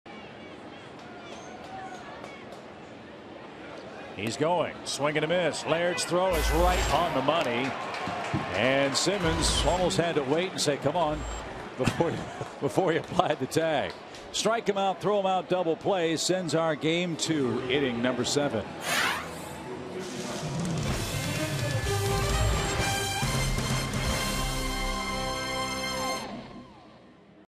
2013/08/21 Braves fan one, nab one
ATL@NYM: Braves strike 'em out, throw 'em out